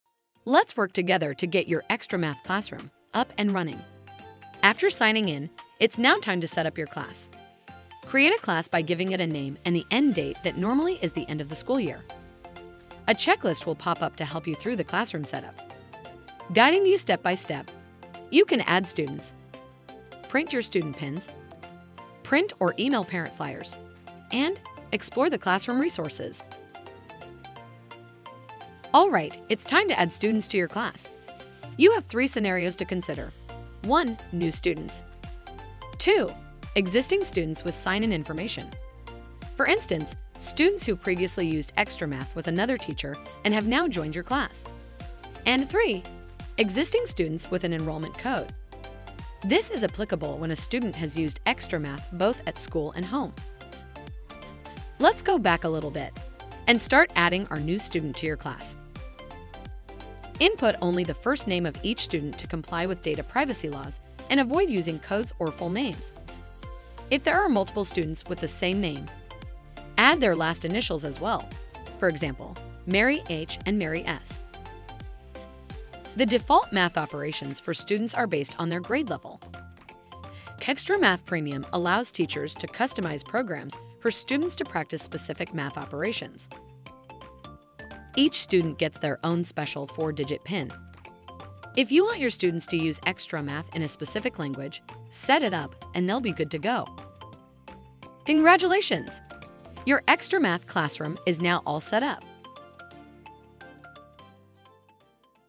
Setting up your class with XtraMath
Our easy-to-follow guide will help you integrate XtraMath into your classroom. We'll walk you through each step, from creating a class and student accounts to customizing settings for your students.